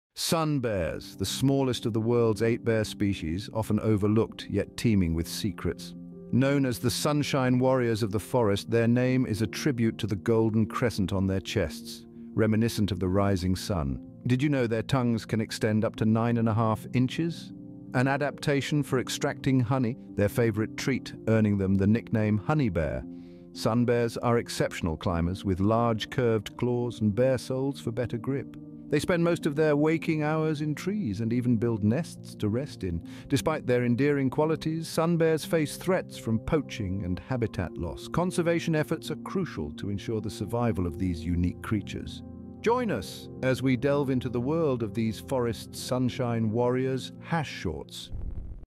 Sun Bear: Sunshine Warrior! ☀️🐻
⁠ "Step into the radiant world of the Sun Bear, the true sunshine warrior of the forest! 🌞🐻 Our short film unveils the untold stories and extraordinary moments of this remarkable creature. Get ready for a journey filled with warmth and wonder—press play now! SunBear SunshineWarrior @WildlifeDailiesChannel